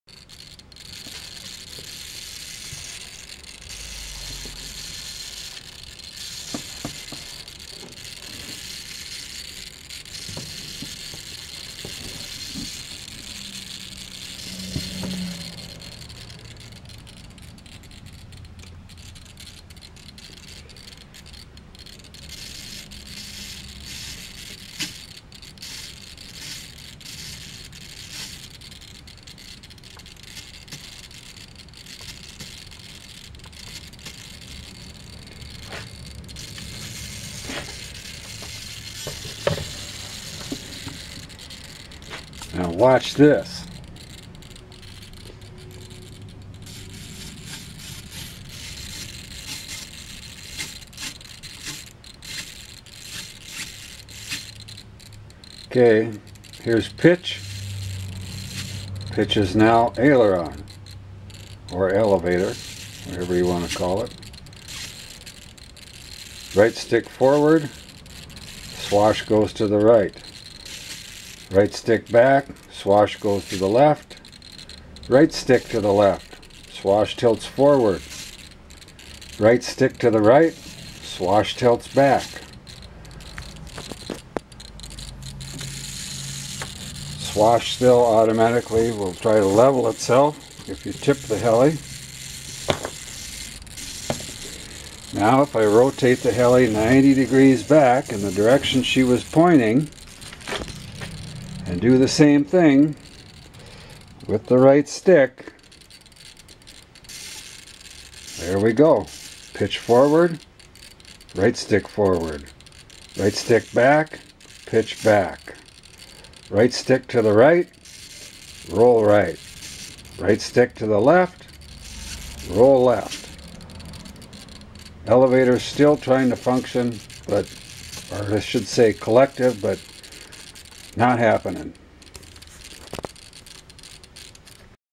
Pixhawk swapping stick comands
heli pointed north all stick inputs normal, rotate heli 90 deg. left or right and same stick commands are now swapped 90 deg.
Only constant is swash will still try to level heli by tilting in proper direction regardless of which direction heli is facing.